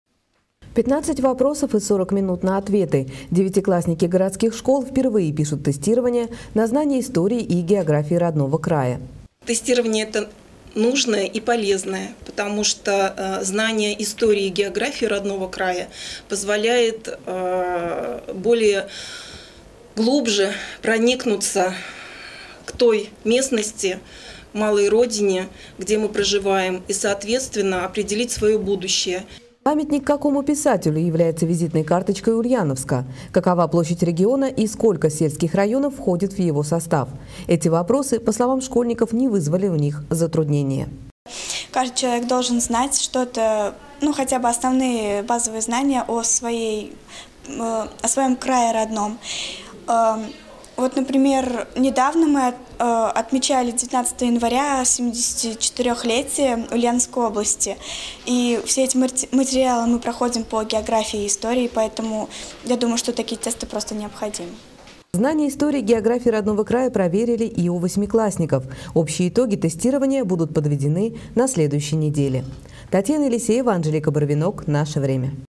Девятиклассников димитровградских школ протестировали на знание истории и географии родного края
15 вопросов и 40 минут на ответы.